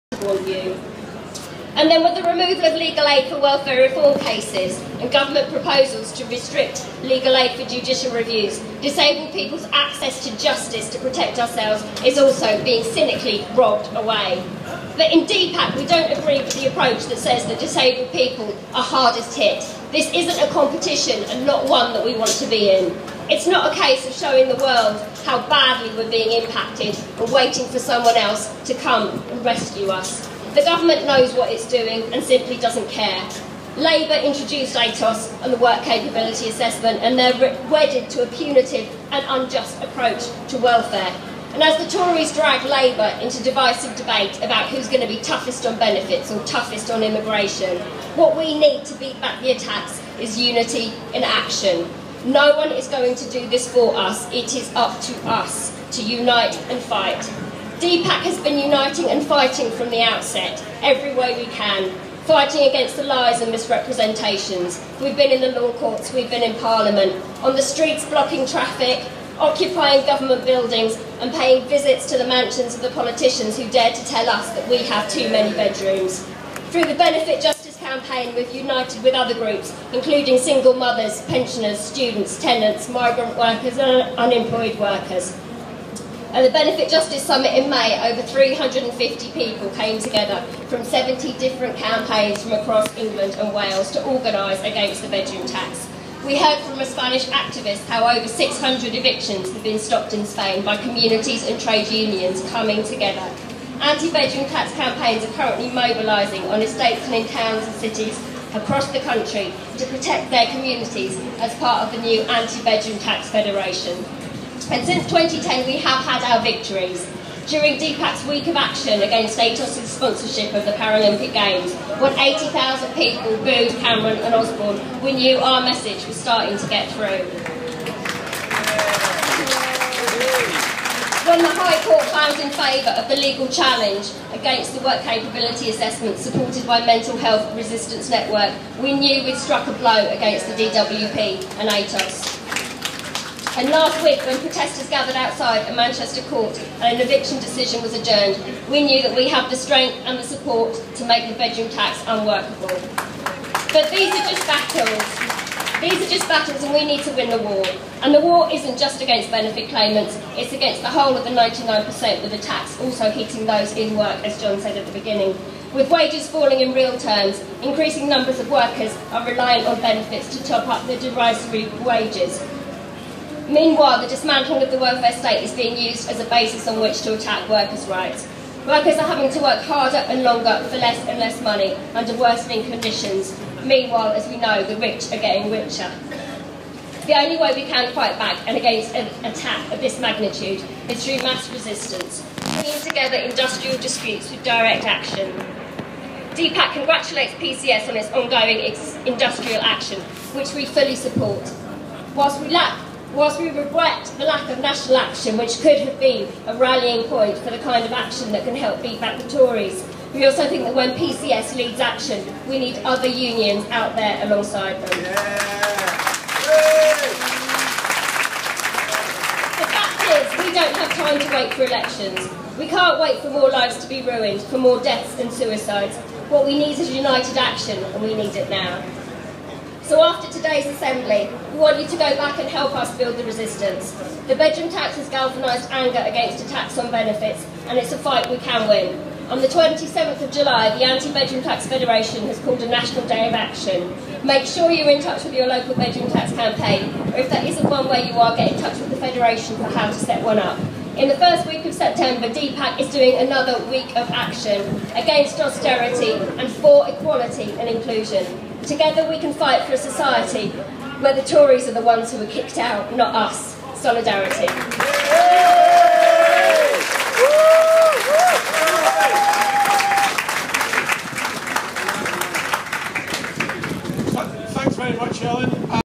Ellen Clifford at People's Assembly
Ellen giving DPAc speech at People's Asembly